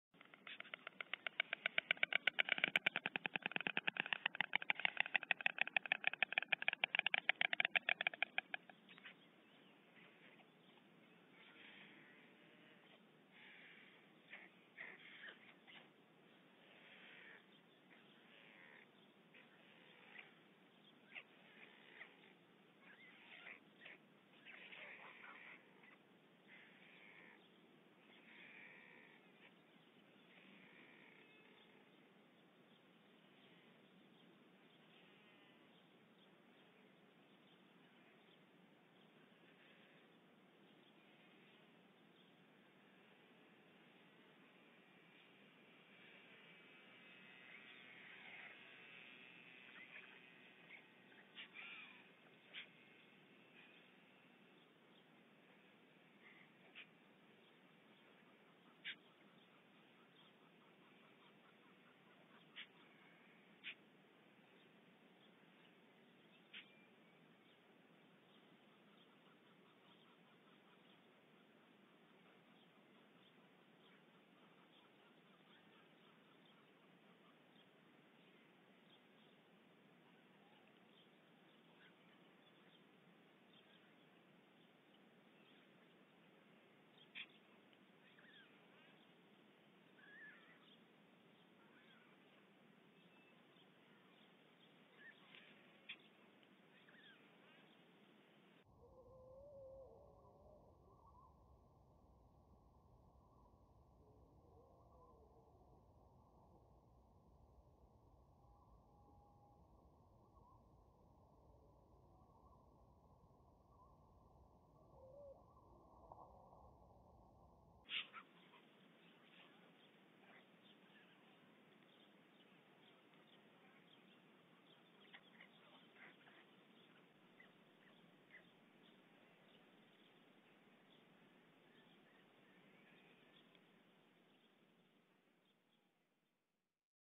INJURED PARENT STORK TAKES FOOD FROM CHICK - NEST OUT OF HELL 2019 SEASON
THIS VIDEO IS FROM THE "NEST OUT OF HELL" 2019 SEASON. INJURED MOM STORK CAN'T HUNT FOR HERSELF, SHE SNAPPED HER FOOT AT THE JOINT. MOM STORK TAKES FOOD OUT OF HER CHICKS MOUTH AFTER CHICK HAS IT HALF WAY DOWN IT'S THROAT.